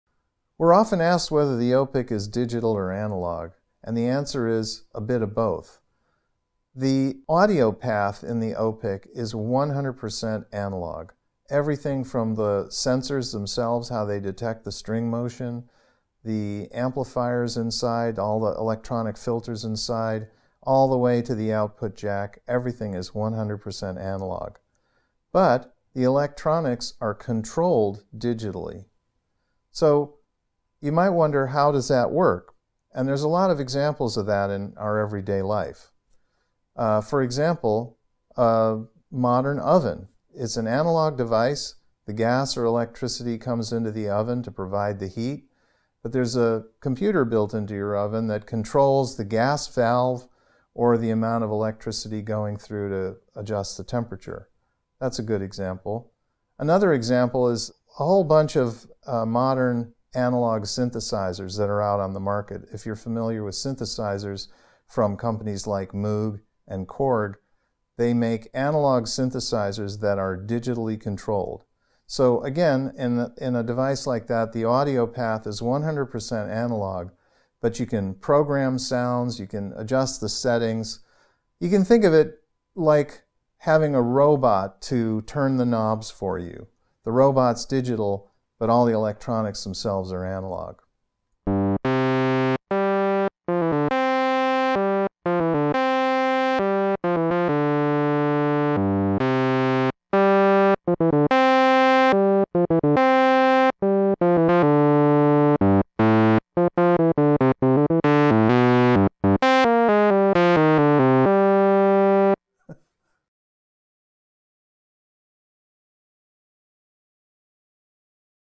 FAQ | Is the ōPik a digital or analog pickup?
The ōPik is truly an end-to-end analog pickup, even though it has digital controls. In this video, Lee explains how it works.